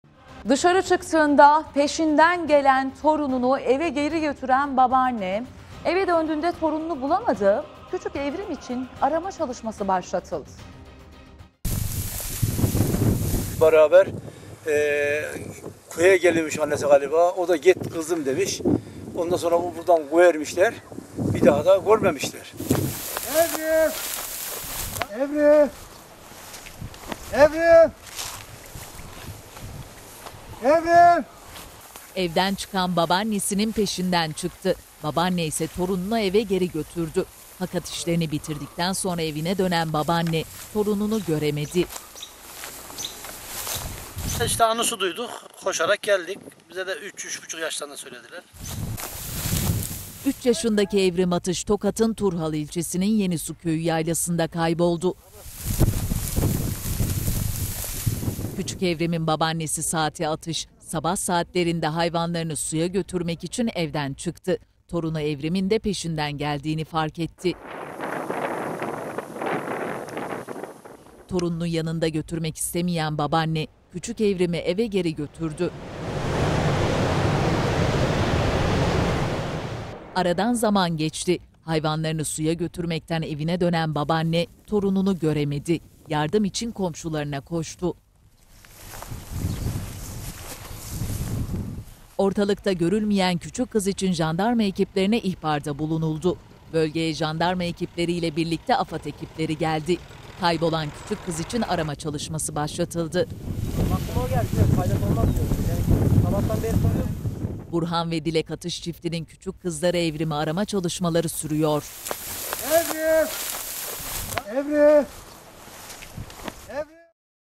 3,5 Yaşındaki Evrim Kayboldu!
Dışarı çıktığında peşinden gelen torununu eve geri götüren babanne, eve döndüğünde torununu bulamadı. Küçük Evrim için arama çalışması başlatıldı.

Küçük Evrim’in babannesi Sati Atış, sabah saatlerine hayvanlarını suya götürmek için evden çıktı.

Torunu Evrim’in de peşinden geldiğini fark etti.
Torununu yanında götürmek istemeyen babaanne, küçük Evrim’i eve geri götürdü.

Fakat eve geri döndüğünde Evrim’i evde bulamadı.
Evrim, Yenisu Köyü Yaylası'nda kayboldu.

TGRT HABER TV HAKKINDA:

● Türkiye'nin kanalıyız...